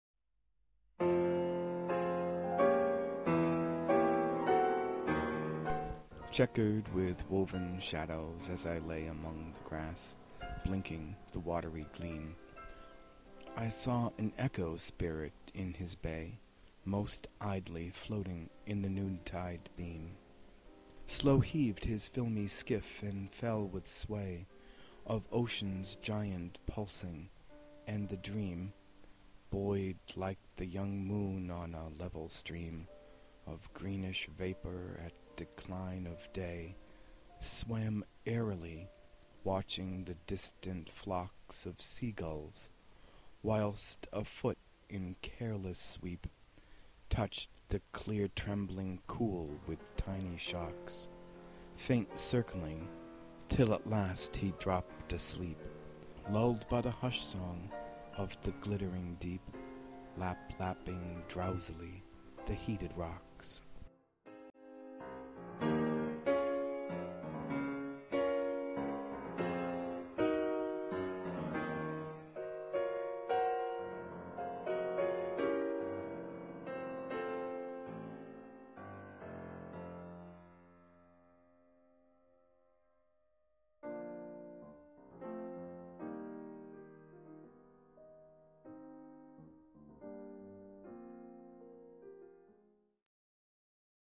William Allingham, A Day-Dream's Reflection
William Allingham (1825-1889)

Born in Ballyshannon, County Donegal, Ireland March 19, 1824, William Allingham worked in a custom house for many years before becoming editor of Fraser's Magazine in 1874.  Dante Gabriel Rossetti was a close friend and illustrated some of his work.

A Day-Dream's Reflection

 Chequer'd with woven shadows as I lay
Among the grass, blinking the watery gleam,
I saw an Echo-Spirit in his bay
Most idly floating in the noontide beam.
Slow heaved his filmy skiff, and fell, with sway
Of ocean's giant pulsing, and the Dream,
Buoyed like the young moon on a level stream
Of greenish vapour at decline of day,
Swam airily, watching the distant flocks
Of sea-gulls, whilst a foot in careless sweep
Touched the clear-trembling cool with tiny shocks,
Faint-circling; till at last he dropt asleep,
Lull'd by the hush-song of the glittering deep,
Lap-lapping drowsily the heated rocks.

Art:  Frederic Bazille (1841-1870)

Born  December 6, 1841, in Montpellier, France, Jean-Frederic Bazille went to Paris to study medicine, but left his study to pursue painting against his parent's wishes.  He befriended Claude Monet, Pierre-Auguste Renoir and Alfred Sisley, and became a prominent Impressionist.

Born in Hamburg May 7, 1933, Johannes Brahms spent most of his life in Vienna. His father first taught him to play piano, and from seven years of age he studied with Otto Friedrich Willibald Cossel.  During his teens he played piano in bars that were also brothels.  He worked with prominent performers of his time such as pianist Clara Schumann and violinist Joseph Joachim. He was a perfectionist and destroyed many of his works.